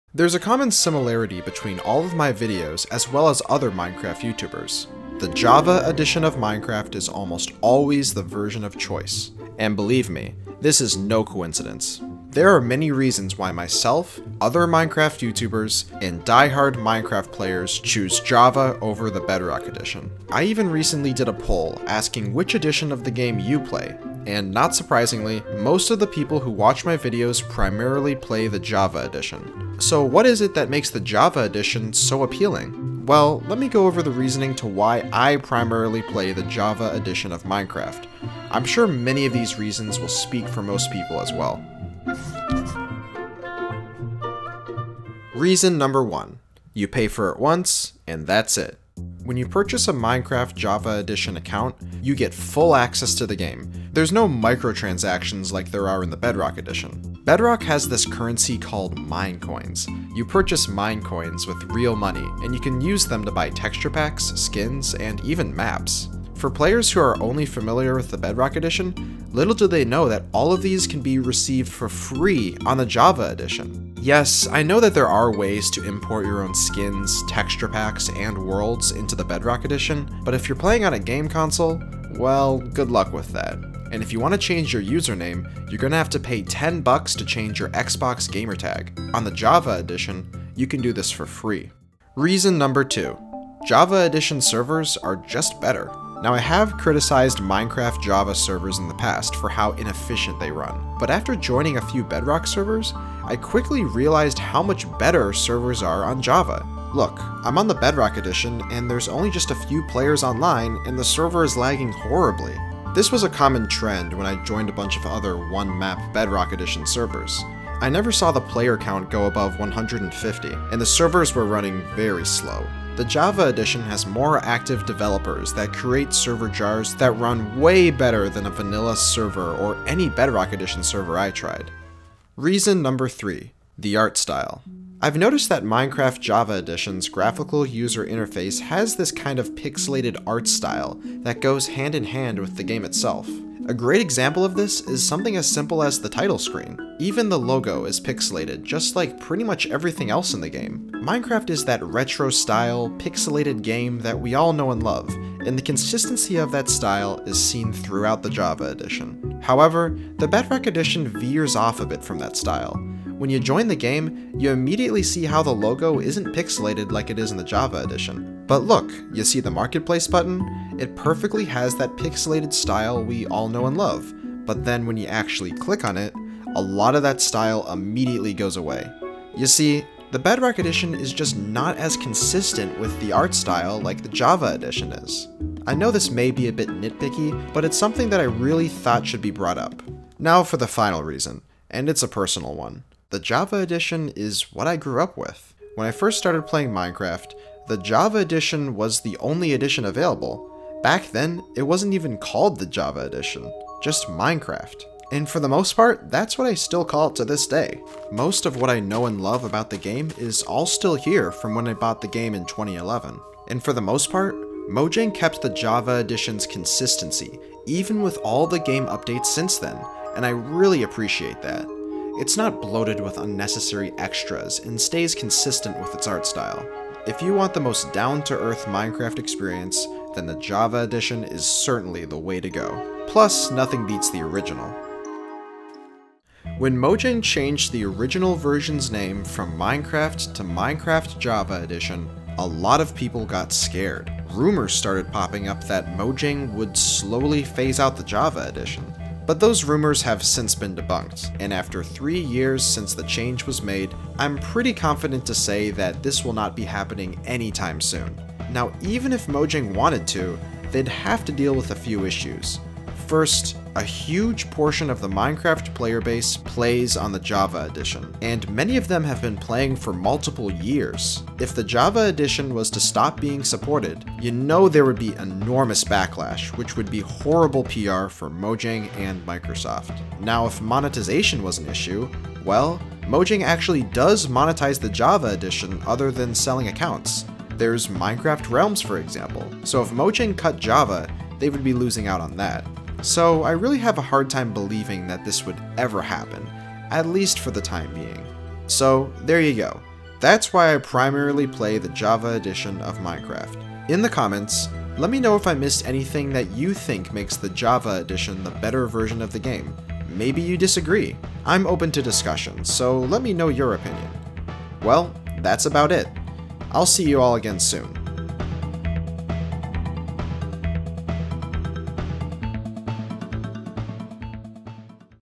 Why I Play the JAVA Edition of Minecraft
Since I bought the game in 2011, I've always stuck with the Java Edition of Minecraft. Let's take a deep look into why I play this specific edition of the game.